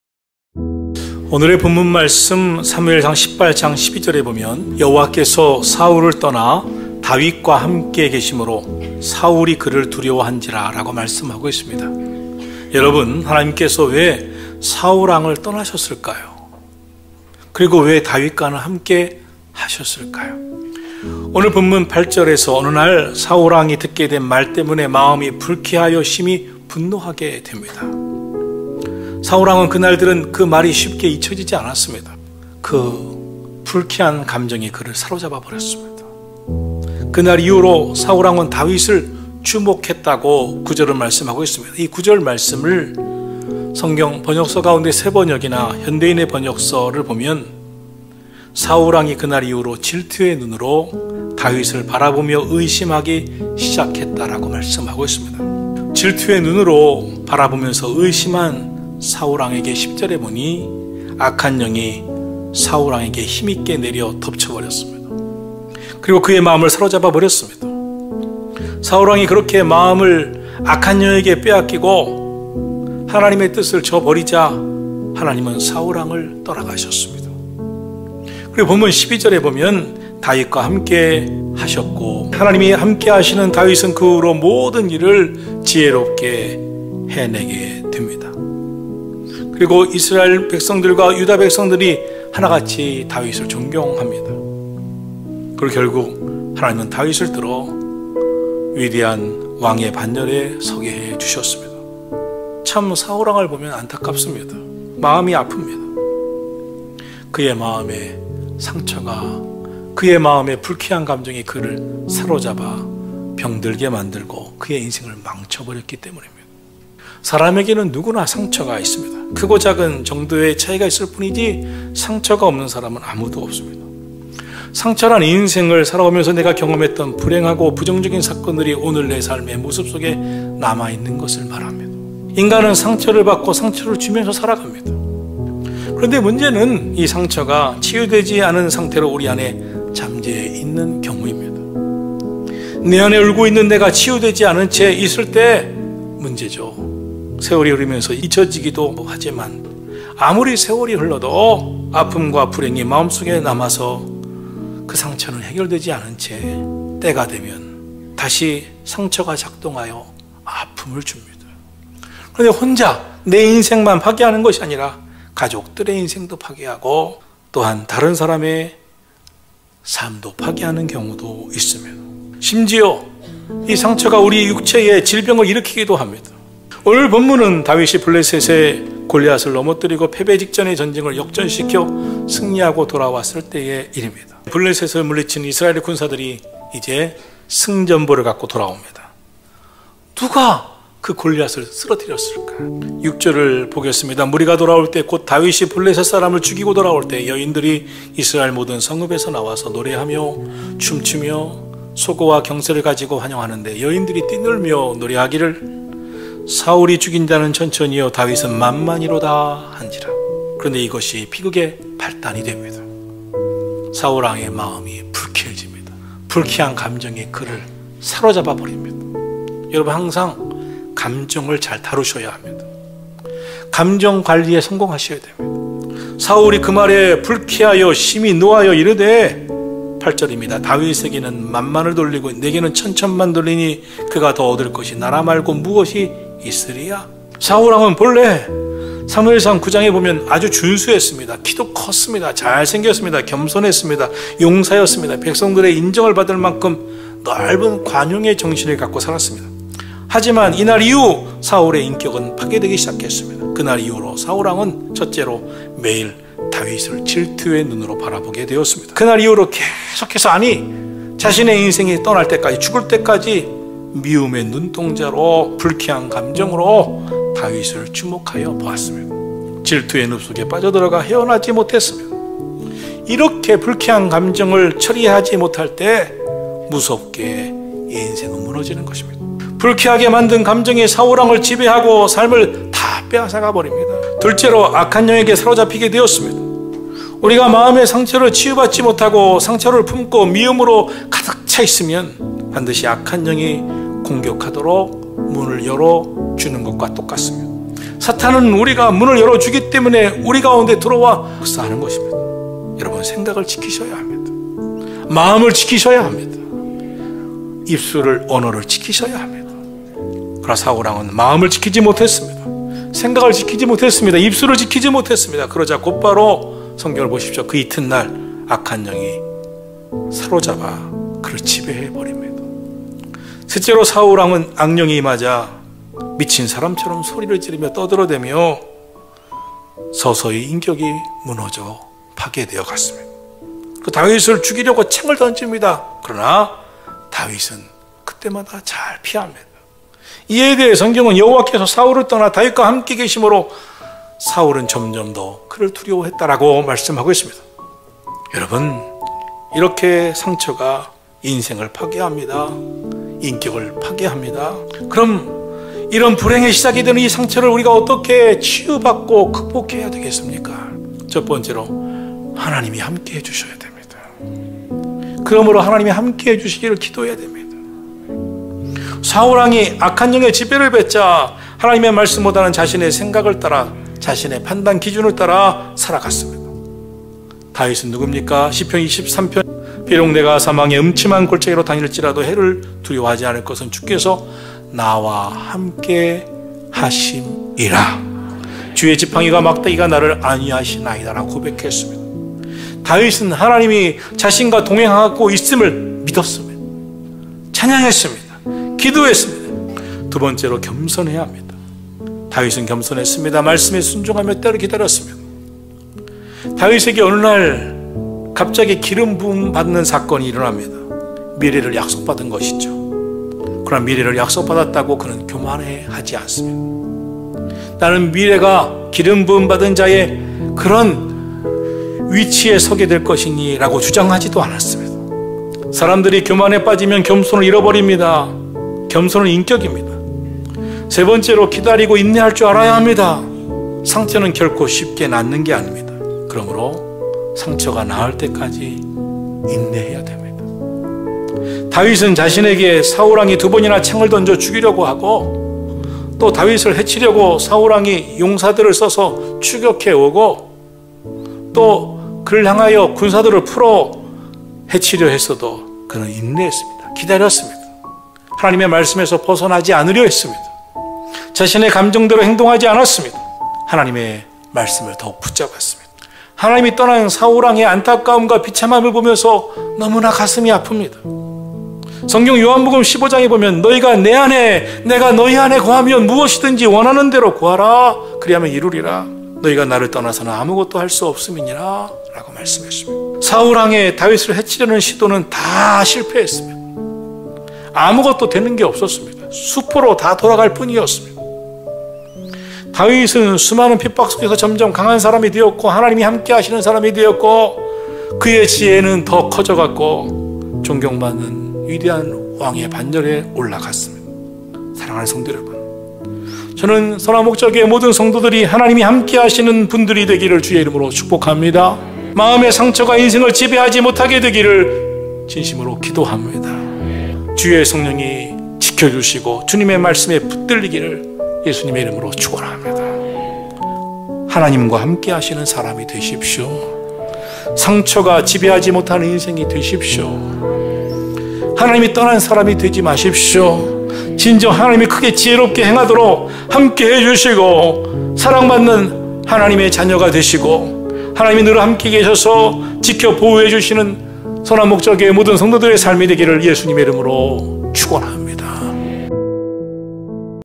[짧은 메시지] 하나님이 떠난 사람과 함께 계신 사람
2월 12일 주일예배 설교
제목: 하나님이 떠난 사람과 함께 계신 사람
본문: 사무엘상 18:7-14